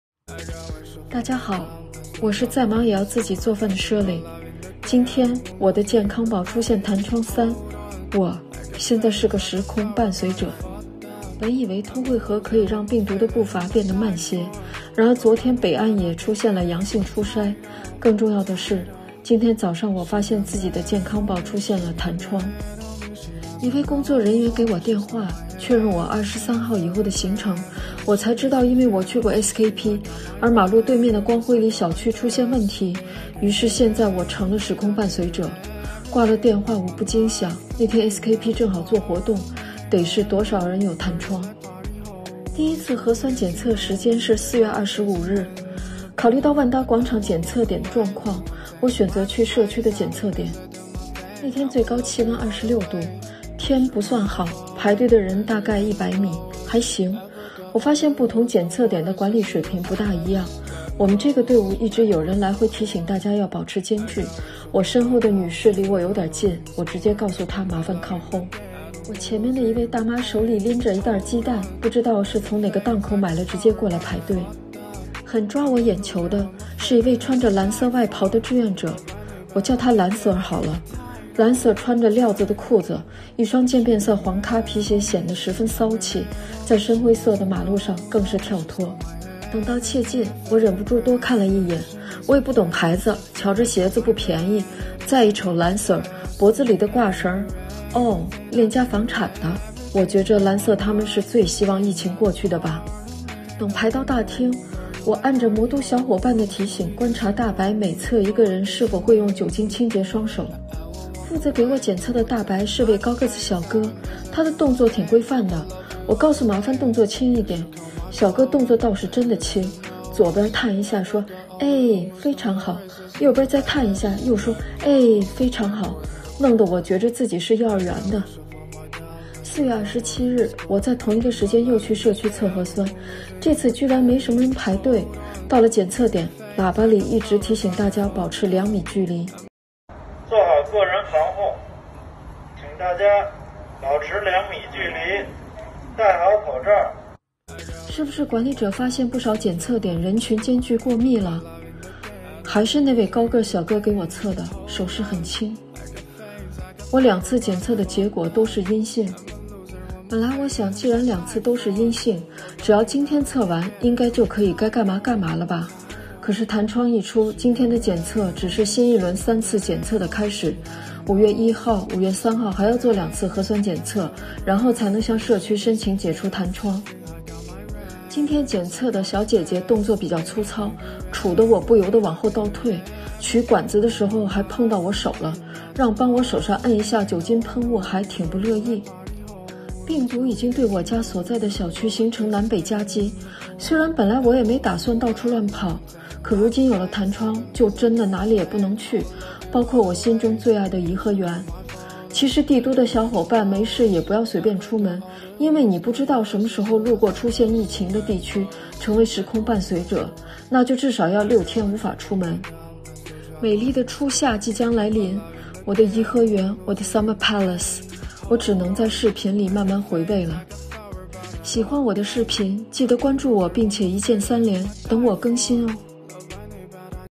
4月24日，我去过SKP，今天我成了时空伴随者
大家好，我是再忙也要自己做饭的Shirley。今天，我的健康宝出现弹窗3， 我，现在是时空伴随者。

本以为通惠河可以让病毒的步伐变得慢些，然而昨天北岸也出现了阳性初筛。更重要的是，今天早上我发现自己的健康宝出现弹窗。

一位工作人员给我电话，确认我23号以后的行程，我才知道因为我去过SKP，而马路对面的光辉里小区出现问题，于是我成了时空伴随者。挂了电话我不禁想，那天SKP正好做活动，得是有多少弹窗3？

说说我已经做的3次核酸检测吧。

Round #1
第一次核酸检测时间是4月25日。考虑到万达广场检测点的状况，我选择去社区的检测点。

那天最高气温26℃，天不算好，排队的人大概100米，还行。我发现不同检测点的管理水平不大一样，我们这个队伍一直有人来回提醒大家要保持间距。我身后的女士离我有点近，我直接告诉她麻烦靠后。

我前面的一位大妈手里拎着一袋鸡蛋，不知道是从哪个档口买了直接过来排队，

很抓我眼球的，是一位穿着蓝色外袍的志愿者，我叫他“蓝Sir”好了。蓝Sir穿着料子的裤子，一双渐变色黄咖皮鞋显得十分骚气，在深灰色的马路上，更是跳脱。

等到切近，我忍不住多看了一眼，我也不懂牌子，瞧着鞋子不便宜，再一瞅蓝Sir脖子里的挂绳，哦，链家房产的。我觉着，蓝Sir他们是最希望疫情过去的吧。

等排到大厅，我按着魔都小伙伴的提醒，观察大白每测一个人是否会用酒精清洁双手。

负责给我检测的大白是位高个子小哥，他的动作挺规范的，我告诉麻烦动作轻一点。小哥动作倒是真的很轻，左边探一下，说“唉~~，非常好”，右边再探一下，又说“唉~~，非常好”，弄得我觉着自己是幼儿园的。

Round #2
4月27日，我在同一个时间又去社区测核酸，这次居然没什么人排队，到了检测点，喇叭里一直在提醒大家保持2米距离。

是不是管理者发现不少检测点人群间距过密了？还是那位高个小哥给我测的，手势很轻。

我两次检测的结果都是阴性。

Round #3
本来我想着既然两次都是阴性，只要今天测完，应该可以该干嘛干嘛了吧。可是弹窗一出，今天的检测只是新一轮三次检测的开始，5月1号、5月3号还要做2次核酸检测，然后才能向社区申请解除弹窗。

今天检测的小姐姐动作比较粗糙，杵得我人不由的倒退，取管子的时候还碰到我手了，让帮我手上按一下酒精喷雾还挺不乐意。

病毒已经对我家所在的小区形成南北夹击，虽然本来我也没打算到处乱跑，可如今有了弹窗，就真的哪里也不能去，包括我心中最爱的颐和园。其实帝都的小伙伴没事也不要随便出门，因为你不知道什么时候路过出现疫情的地区，成为时空伴随者，那就至少要6天无法出门。

美丽的初夏即将来临，我的颐和园，我的Summer Palace，只能在视频里慢慢回味了。

喜欢我的视频，记得关注我并且一键三连，等哦更新哦。